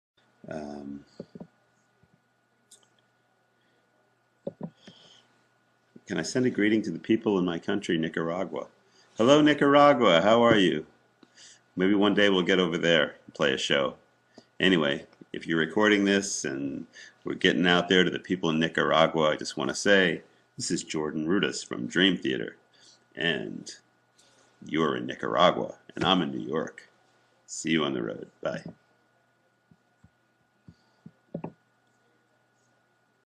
A Greeting to Nicaragua from Jordan Rudess
Jordan Rudess keyboardist from Dream Theater was kind enough to attend my request, so he sent a greeting to fans in Nicaragua.

Thanks Jordan!